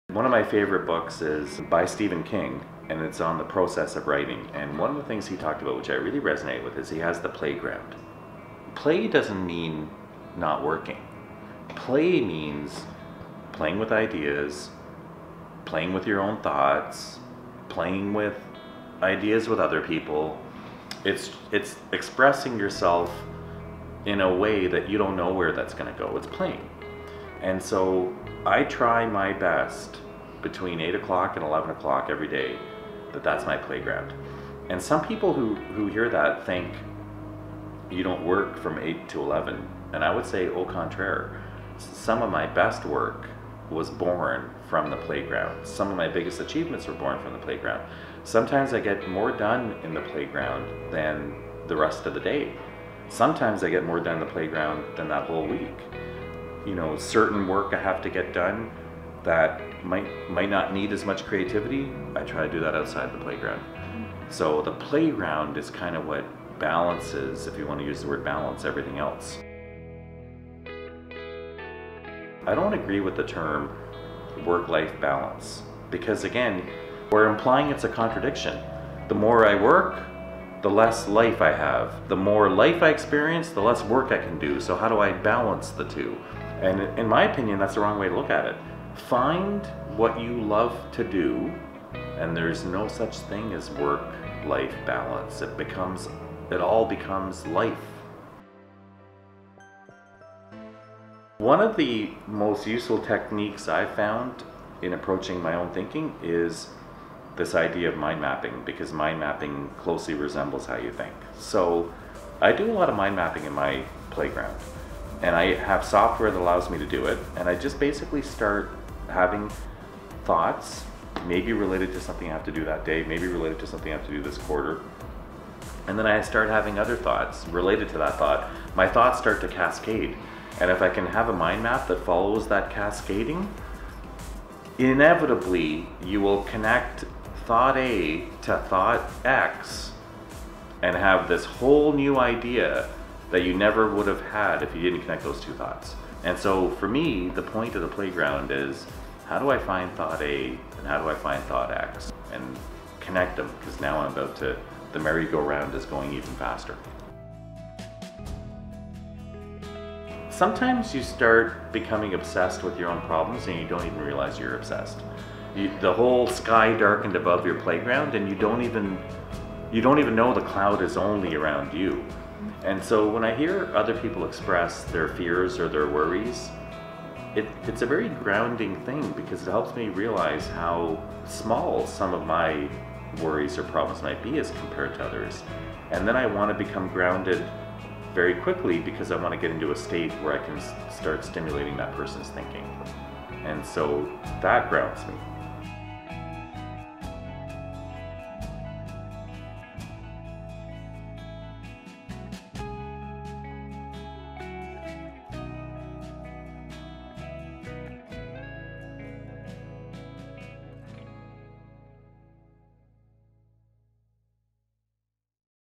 Playgrounds | Omer Aziz
Omer Aziz is a Human Resources Executive who has a unique way of managing his time. For a busy executive, Omer blocks out a period of time everyday to be in the “playground”.